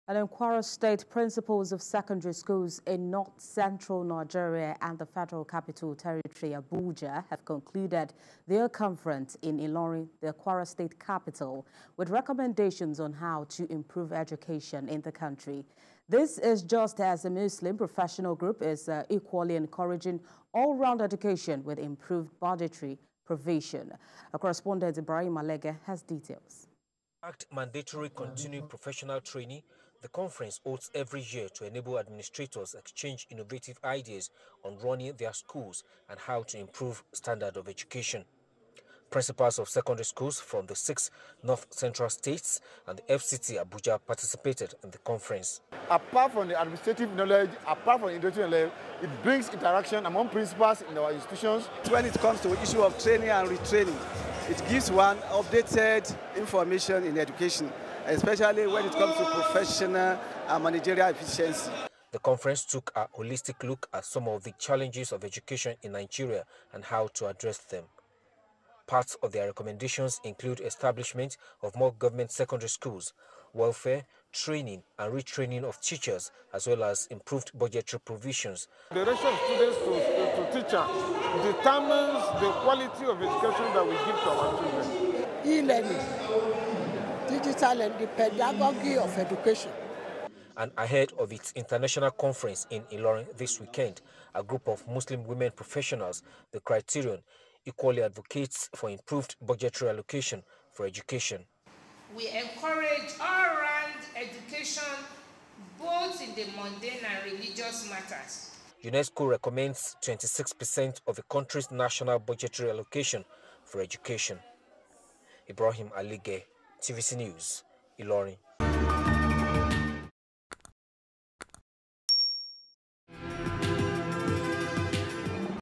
North Central School Principals Hold Conference in Ilorin
Principals of secondary schools in North Central Nigeria and the Federal Capital Territory, Abuja, have concluded their conference in Ilorin, the Kwara state capital with recommendations on how to improve education in the country.
This is just as a Muslim professional group is equally encouraging all round education with improved budgetary provision.